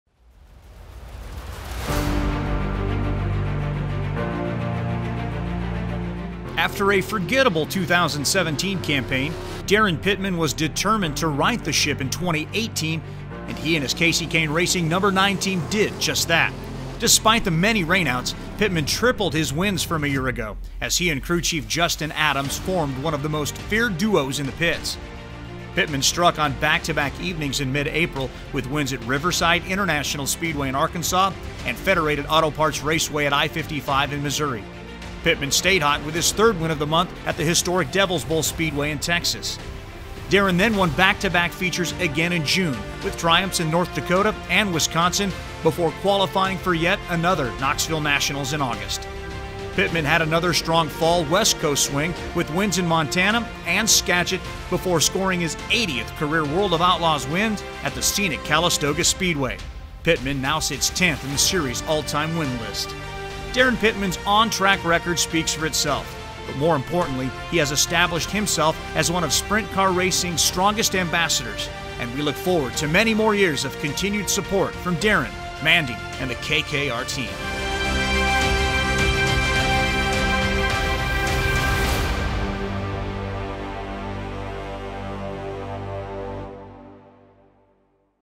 Daryn Pittman | 2018 World of Outlaws Craftsman Sprint Car Series Season In Review
Take a look back at Daryn Pittman’s 2018 Season with the World of Outlaws Craftsman Sprint Car Series!